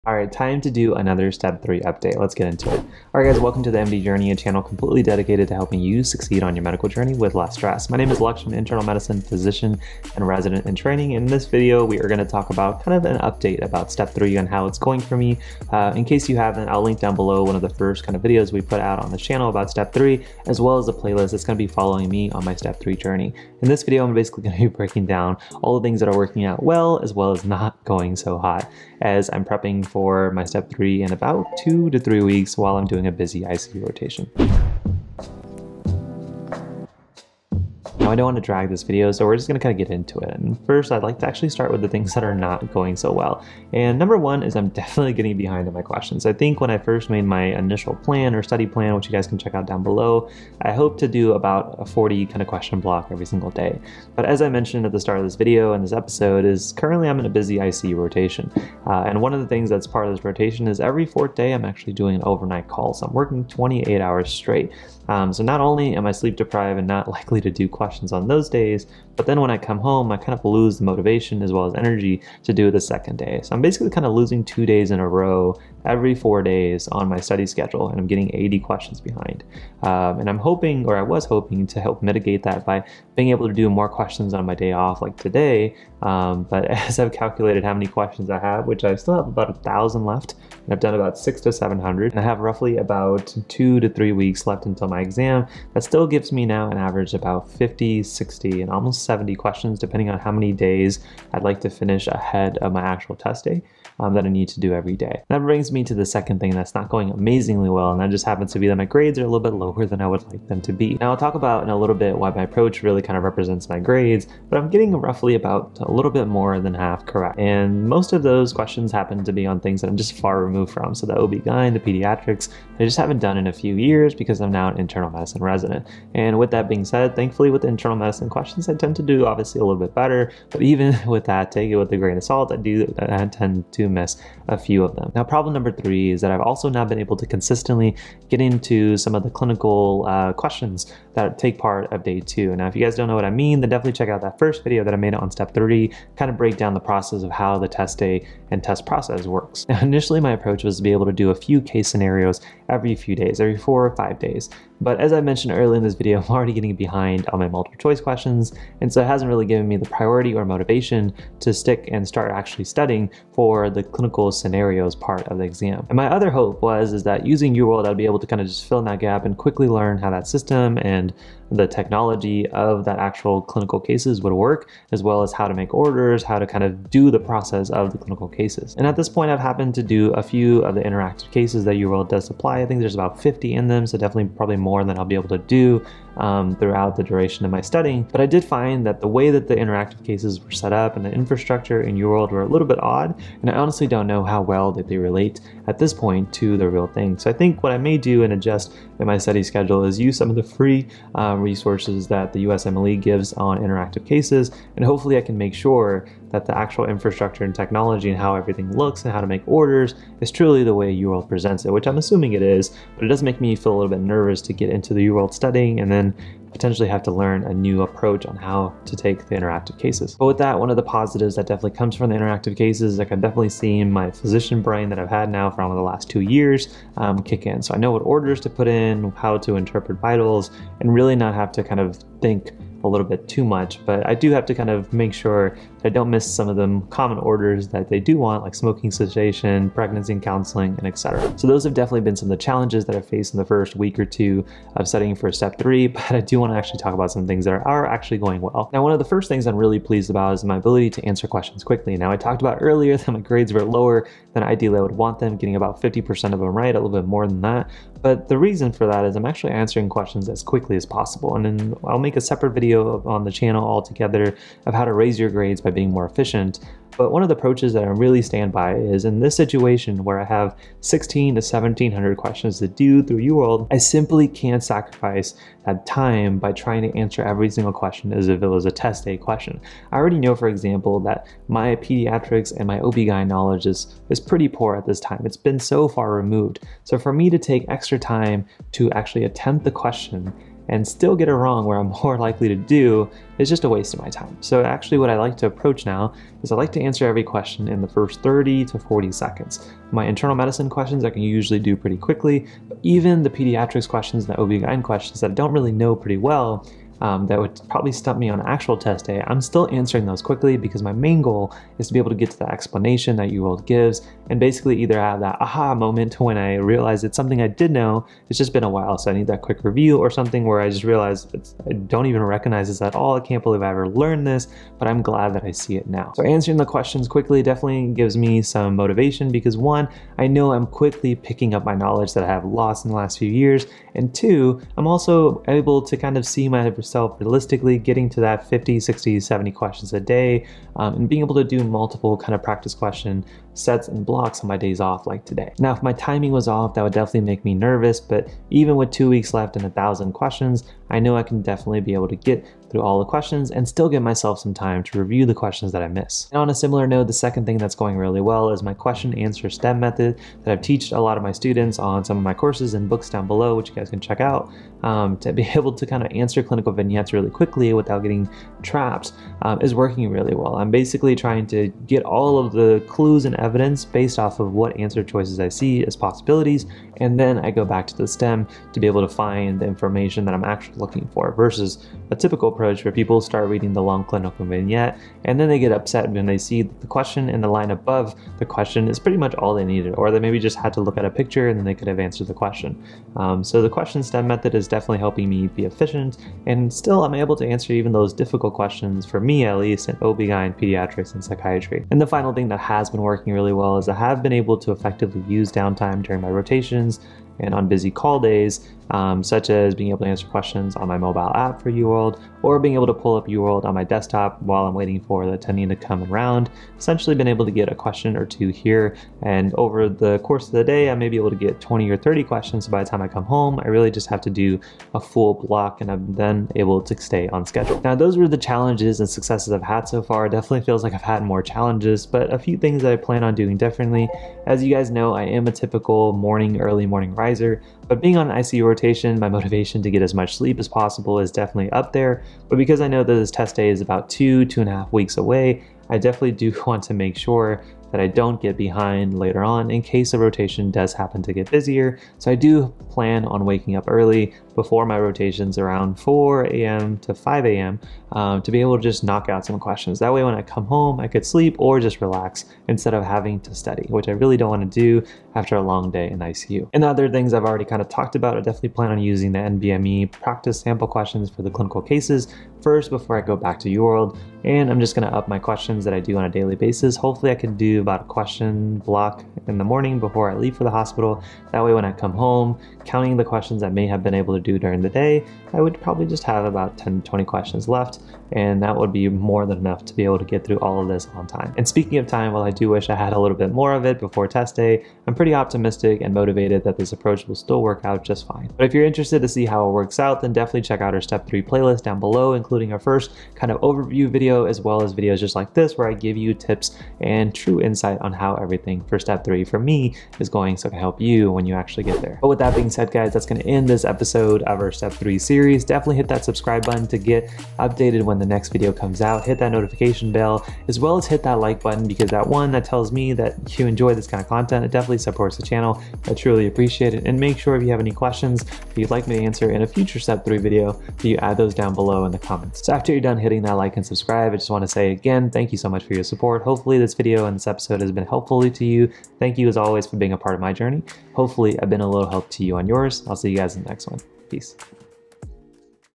USMLE Step 3 - How To Keep Up With Your Study Schedule
How do you keep up with your USMLE Step 3 Schedule?

I am studying and preparing for Step 3 and a lot has changed since my first video and how I planned to tackle it. Hours and in my rotations and time in the hospital has forced me to make changes in my study schedule, number of questions reviewed for UWORLD per day, taking on a new resource and others.

Not everything is going to plan but others have and I want to show premeds and most especially med students and residents prepping for Step 3 that even as your initial study plan doesn’t work, don’t be afraid to adjust as test day approaches.

In this video, I give you an update on Step 3, what’s working and is not working, and what my new plan looks like.

What are you doing to prepare for the test!? Is there something you’d do differently!?

How To Get Better Grades In Medical School Using Just One Tip

TMJ Podcast:⤵️
Popular Resources:⤵️